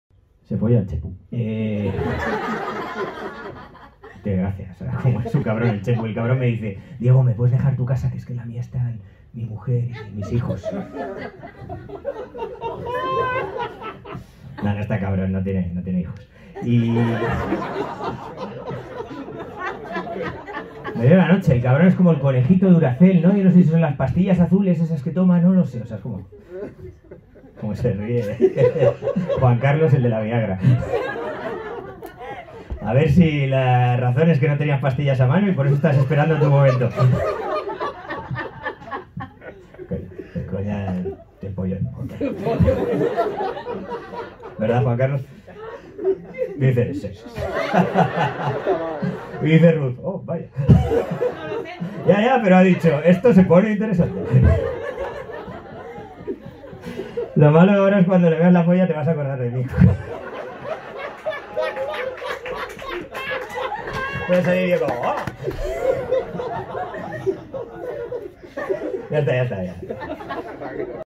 Diego Daño. Monólogo Cupido Debe Morir. Improvisación con público en Madrid el 23 de octubre.
Improvisación con una pareja en su segunda cita.